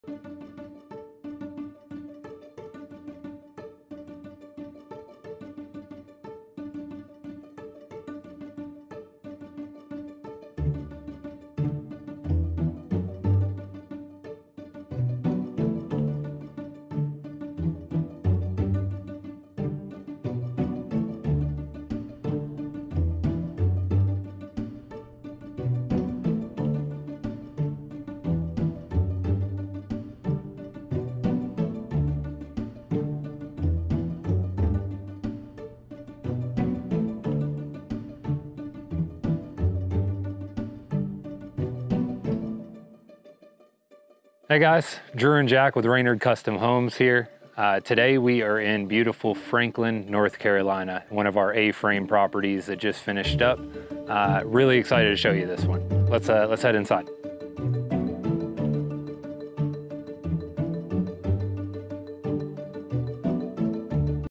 Tour our Stunning A-Frame Property in Franklin, NC | Raynard Custom Homes
Explore the breathtaking A-frame design of our recently completed property in Franklin, North Carolina. Join us as we take you inside for a detailed tour and reveal the unique features that make this home a true standout. Don't miss out on this opportunity to witness the artistry and craftsmanship of Raynard Custom Homes.